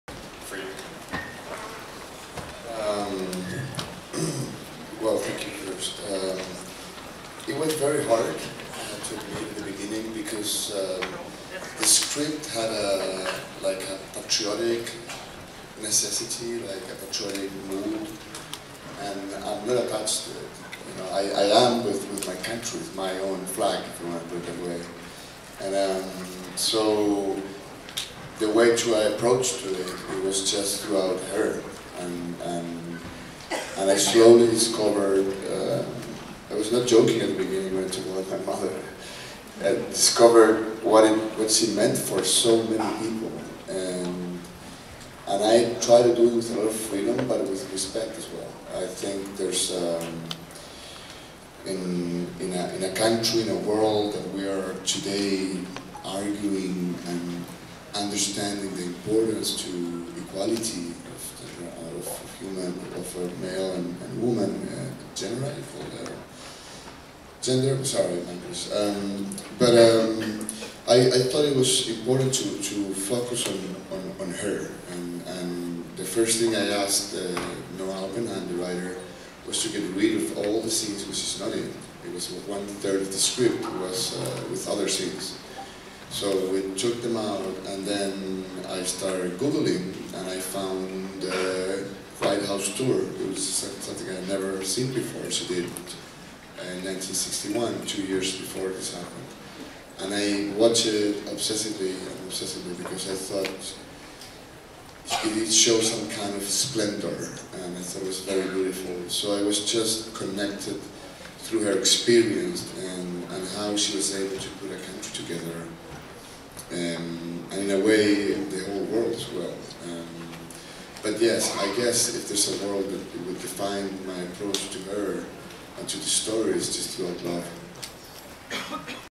TIFF: Jackie's Pablo Larrain
Jackie director Pablo Larrain discusses discovering the First Lady Jacqueline Kennedy as a non-American.